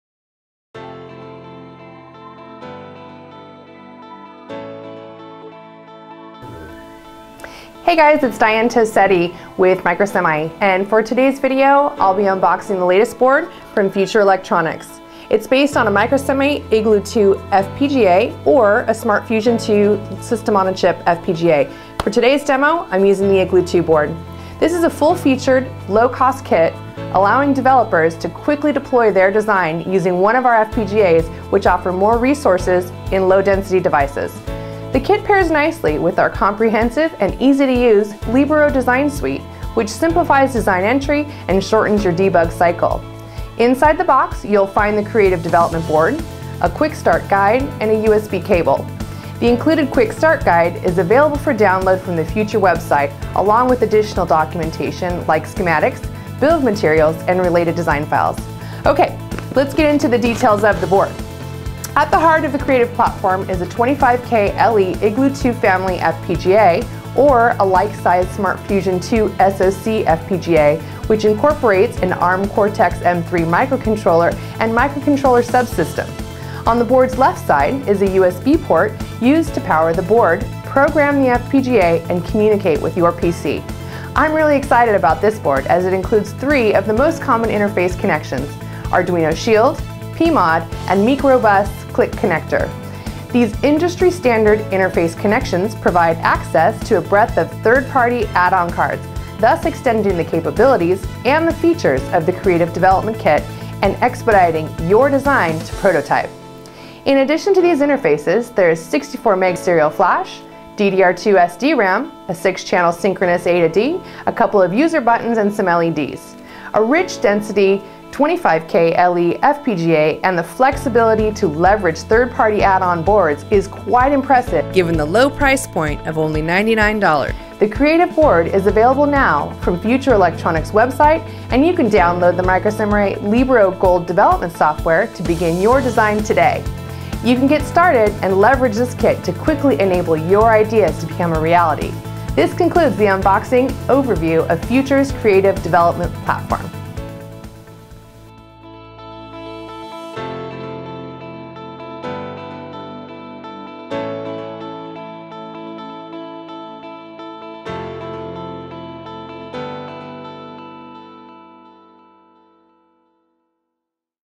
Microsemi: Unboxing the Future Electronics Creative Board
A quick look at the Future Electronics "Creative Development Board" featuring Microsemi's Igloo®2 FPGA (or Microsemi's SmartFusion®2 SoC FPGA).  ... 10.2017 update.